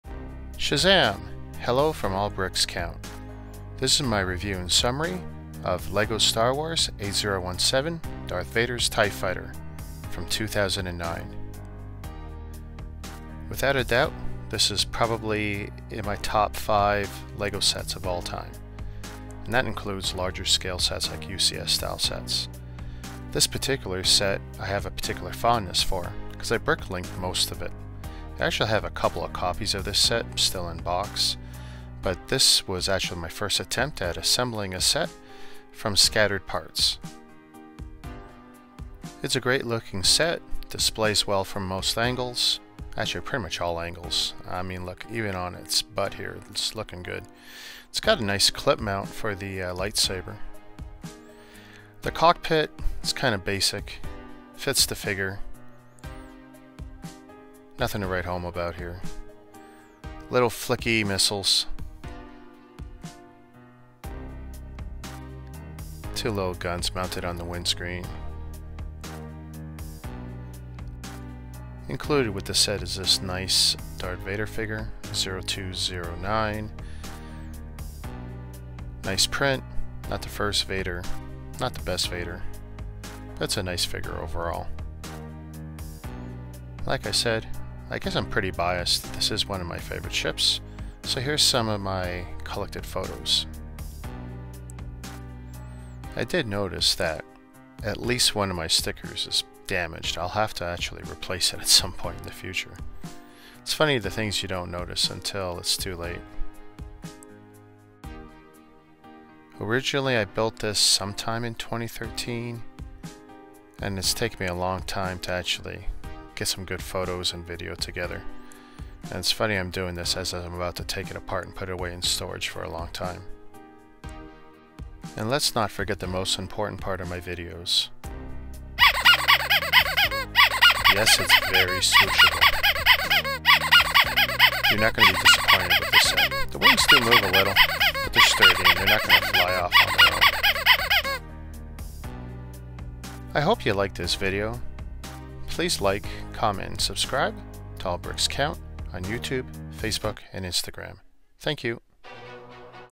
Lego 8017 Darth Vader’s TIE Fighter 2009 Star Wars
This is my review and summary for one of my all time favorite Lego Star Wars sets.. 8017 from 2009.

Darth Vader’s TIE Fighter.

I built this sets years ago, and I’m pretty sure this was the first set I cobbled together from misc bricklink orders.

All Bricks Count • News • Reviews • Live Builds • Brick Hauls • .. and my visual archive/inventory